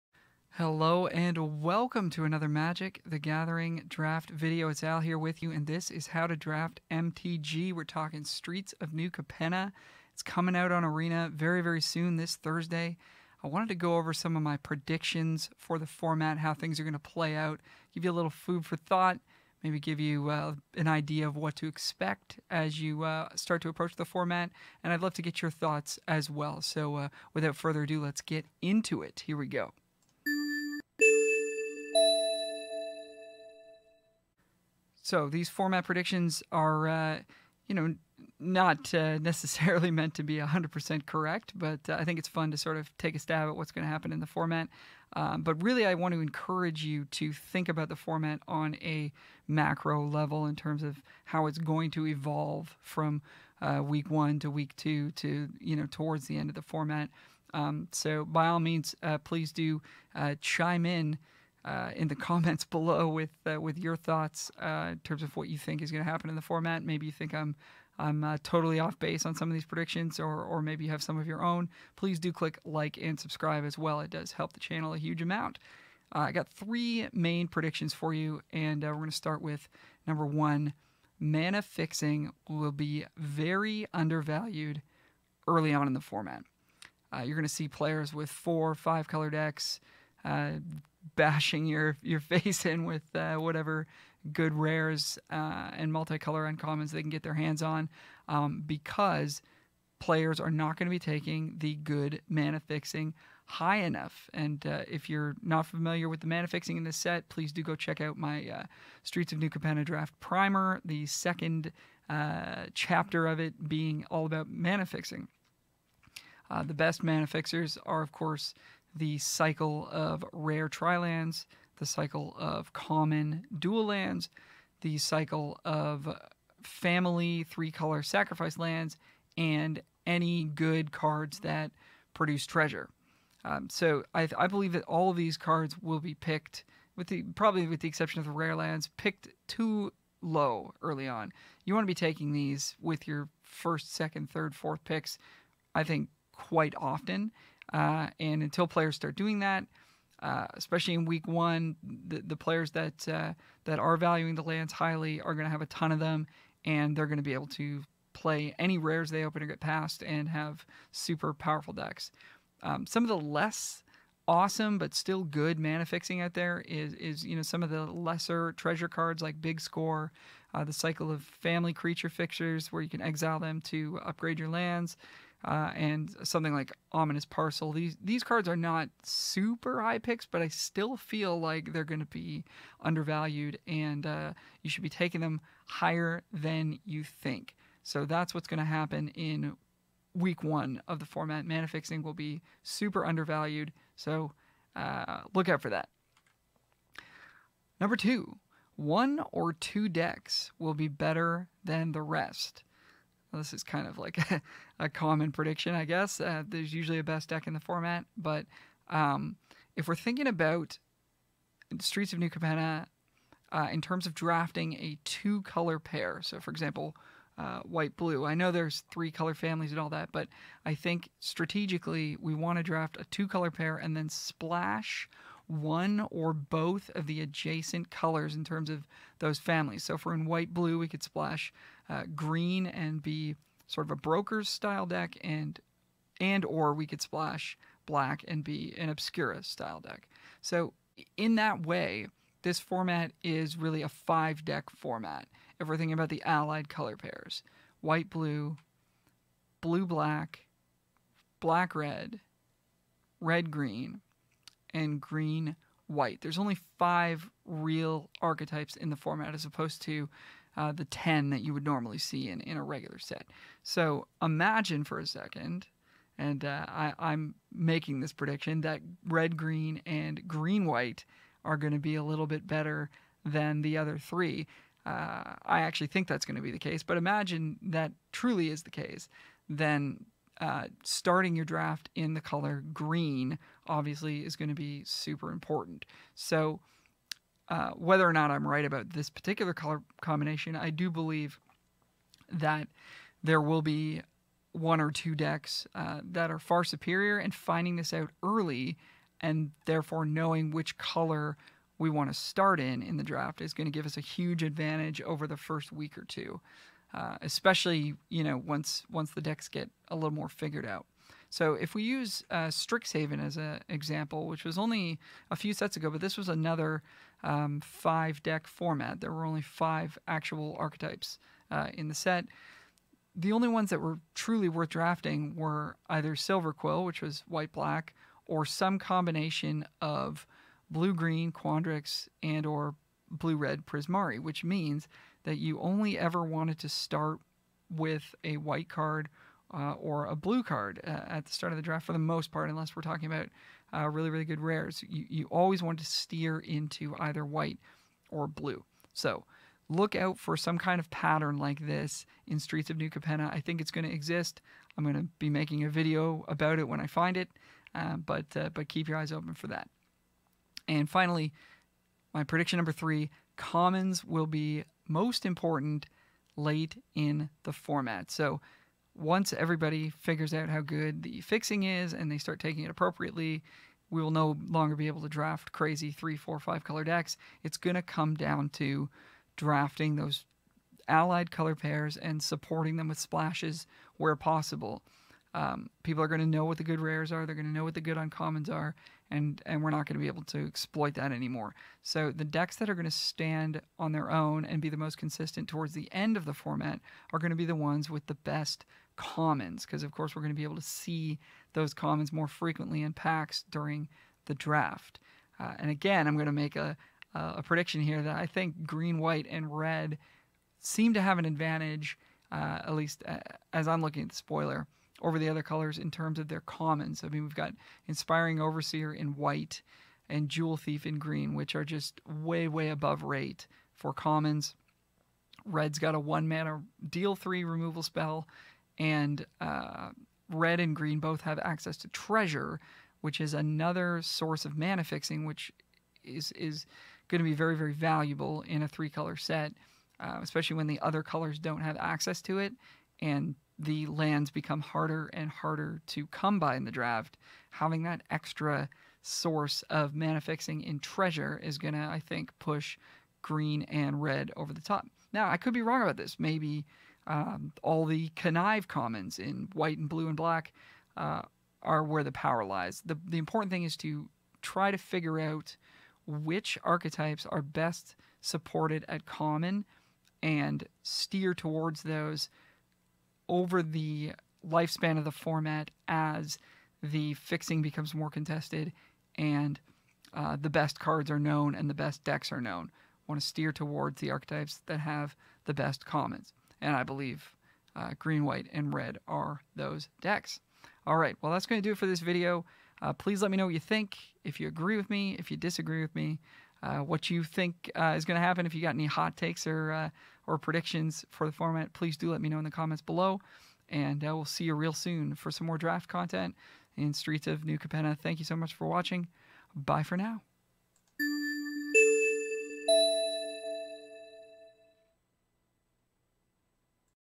Three Predictions for Streets of New Capenna Draft! MTG Limited Guide.
My predictions on how the Magic: The Gathering Streets of New Capenna Draft format will play out over the coming weeks!